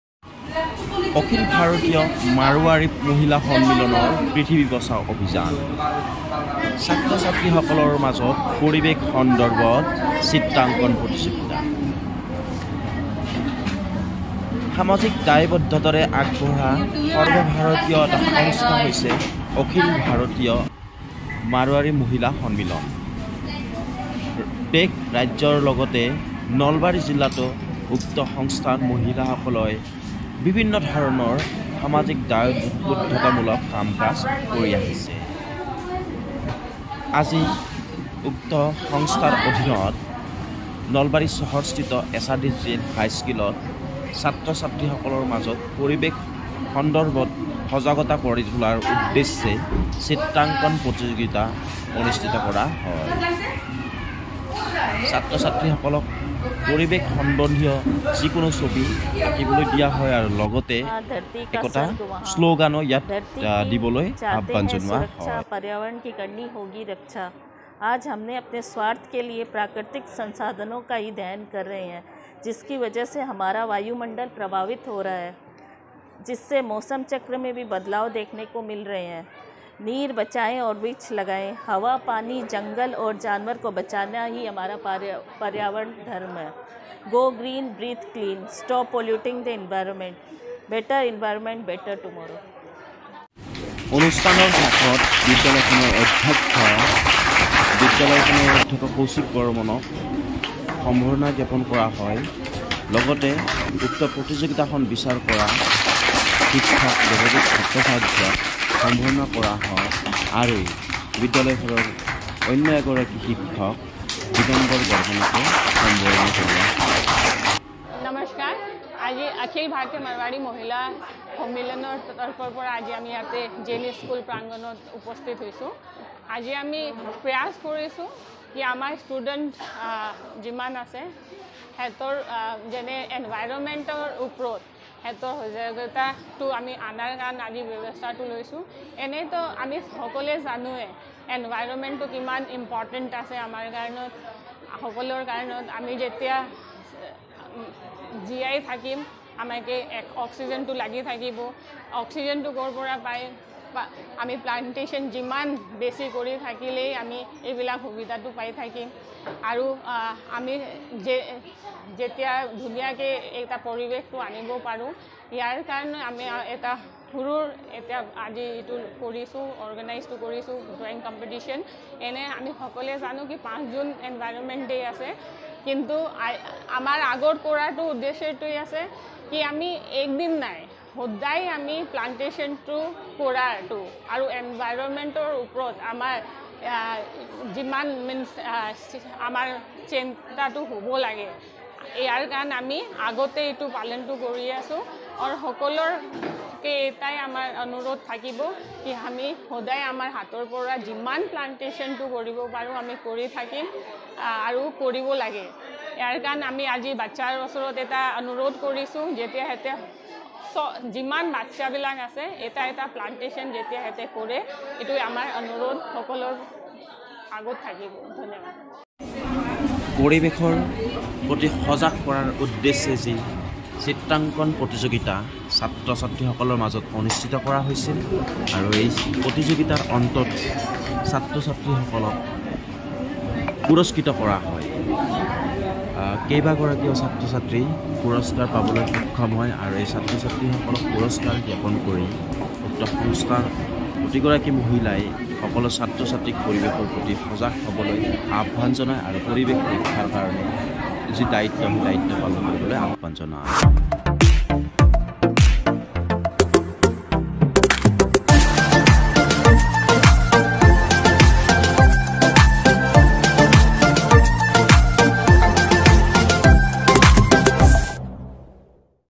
অখিল ভাৰতীয় মাৰোৱাৰী মহিলা সন্মিলনৰ পৃথিৱী বচাও অভিযান।
নলবাৰীত অখিল ভাৰতীয় মাৰোৱাৰী মহিলা সন্মিলনৰ পৃথিৱী বচাও অভিযান।।পৰিৱেশৰ প্ৰতি সজাগতা বৃদ্ধিৰ বাবে ছাত্ৰ-ছাত্ৰী সকলৰ মাজত চিত্ৰাংকন প্ৰতিযোগিতা অনুষ্ঠিত কৰা হয়।। শনিবাৰে নলবাৰী চহৰৰ এছ আৰ দি জৈন হাইস্কুলত অনুষ্ঠিত হয় প্ৰতিযোগিতাখন।